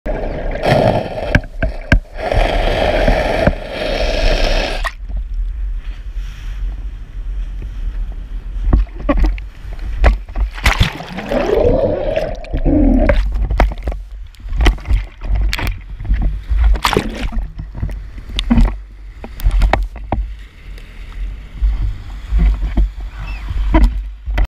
2022 Bisbee's East Cape Offshore | El Suertudo | Striped Marlin
Angler Chris Kirkley catches a Striped Marlin at the 2022 Bisbee's East Cape Offshore.